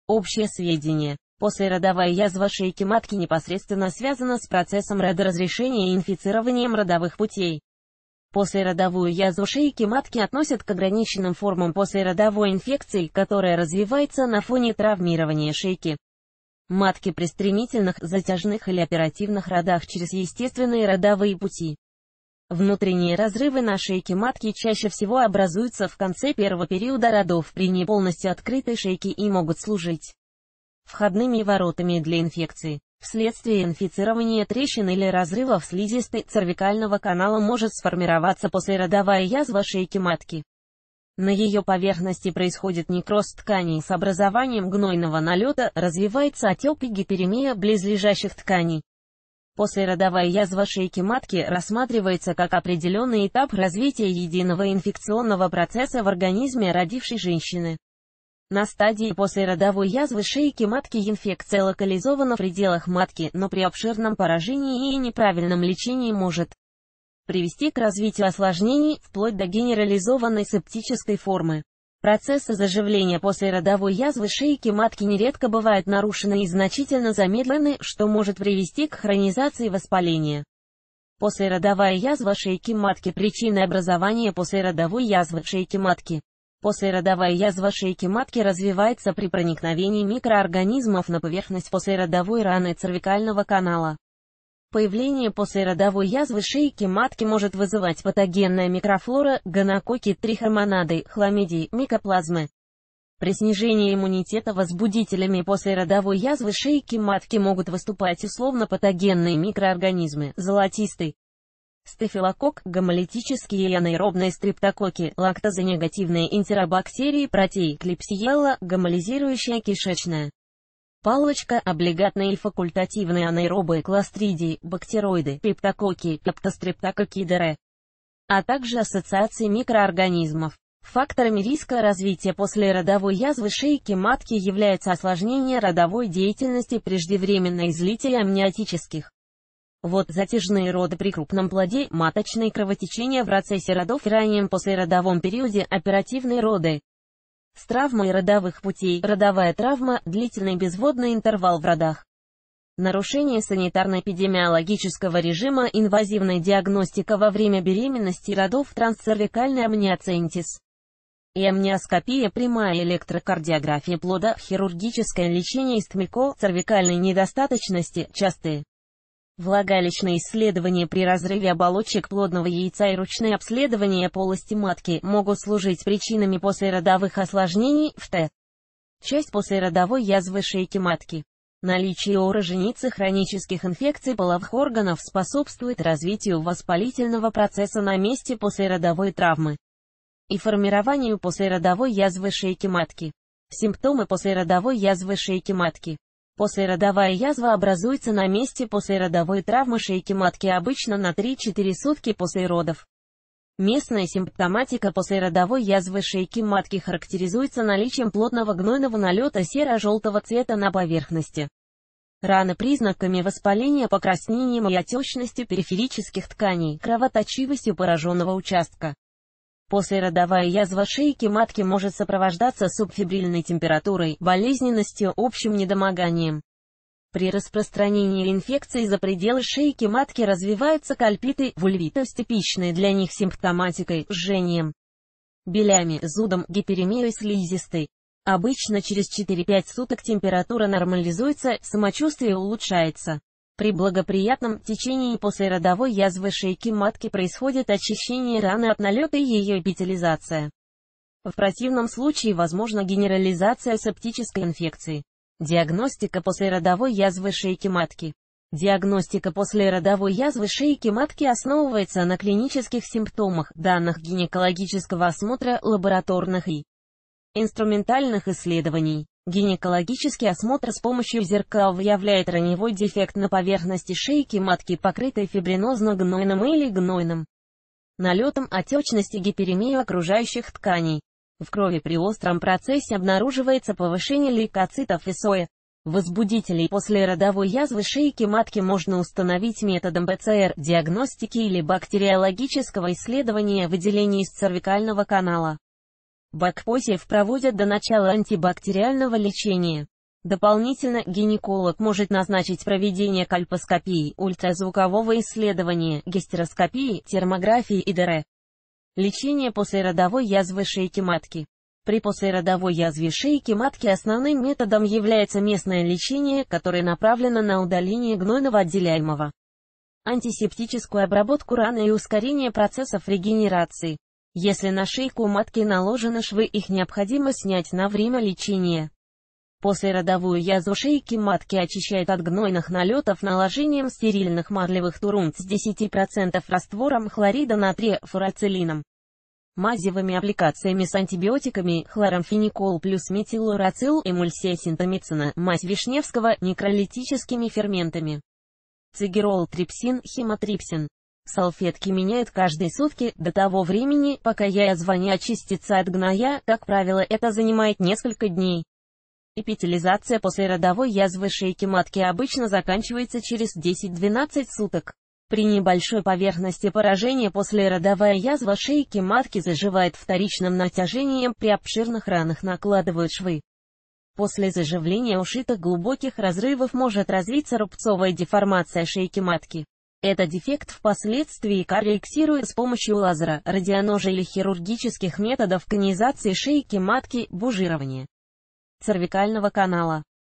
Послеродовая язва шейки матки
Послеродовая язва шейки матки. Симптомы. Диагностика. Что делать при диагнозе послеродовая язва шейки матки. Консервативное лечение и операции. Платные и бесплатные клиники, в которых лечится послеродовая язва шейки матки.